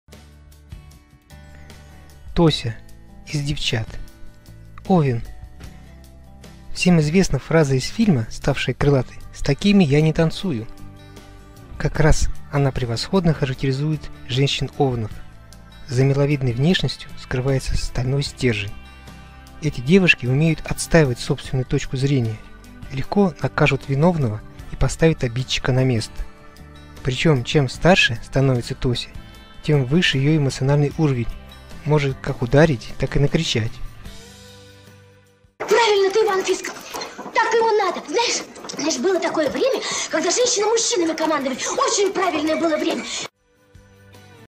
Тося из Девчат Звездная роль и их знаки Зодиака! Актеры и роли
Вряд ли в мире есть люди, которым не нравится смотреть советские картины. И если они все же встречаются, то они просто их не видели. Отличная актерская игра, продуманные герои, безупречные сценарии - эти моменты заставляют нас неоднократно возвращаться к этим шедеврам.
Кстати, вы когда-нибудь пробовали сравнивать себя с главными героинями советских фильмов? например, с Надей ("Ирония судьбы") или с Ниной ("Белые пленницы")? вы знаете, что у вас есть сходство с этими персонажами по гороскопу;